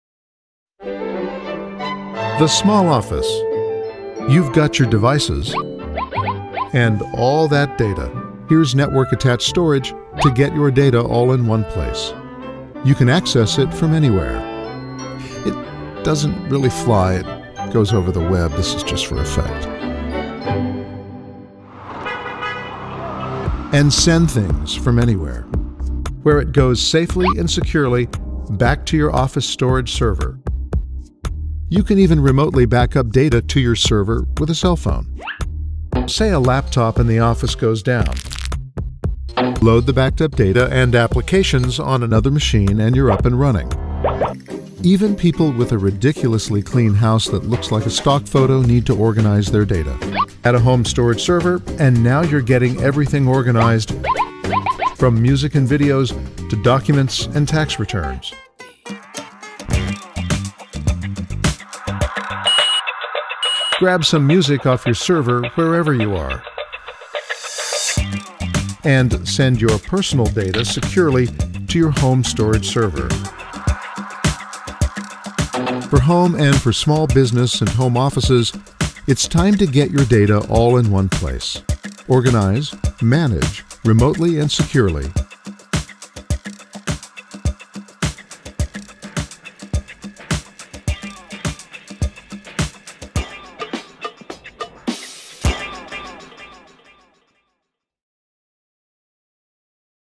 NAS: Storage for Home and Small Office_tc
This video shows how a Network Attached Storage (NAS) solution with Intel® processors can help save time and increase productivity for small business and help organize, share and back-up your data at home. Simply dumping data onto external drives is no longer considered a good practice. Network Attached Storage (NAS) is surprisingly affordable and allows people to actually manage the data they store.